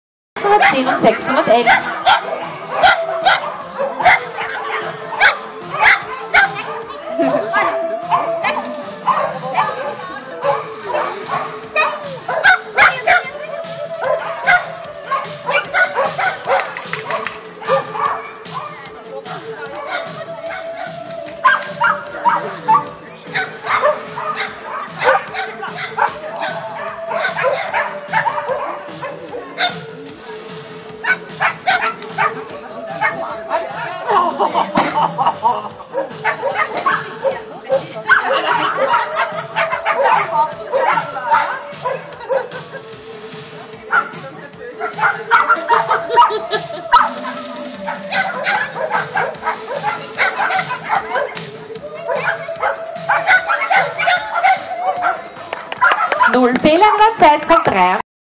Blue Danube Trophy - Zenit Agility 2. place
Blue Danube Trophy - 2013 Korneuburg
Herendy Veronika - Zenit agility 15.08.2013
2.place